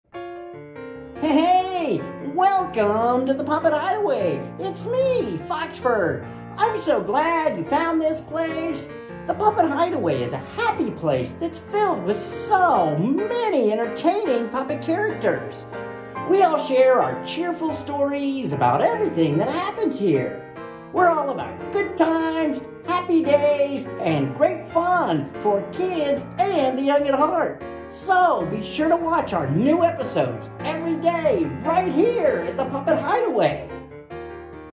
Foxford Introduction To The Puppet Hideaway with Eric Thomsen
Foxford welcomes you to The Puppet Hideaway, a happy place filled with many entertaining puppet characters. We all share our cheerful stories about everything that happens here. We're all about good times, happy days, and great fun for kids and the young at heart.